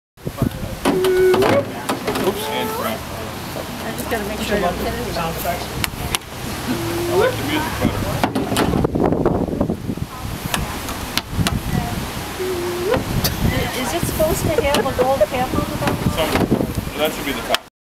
Warriors' Watch Riders of Illinois flag assembly day.
Warriors' Watch Riders of Illinois flag assembly day. 250 flags were assembled for use in Welcome Homes, troop sendoffs and other important WWR events.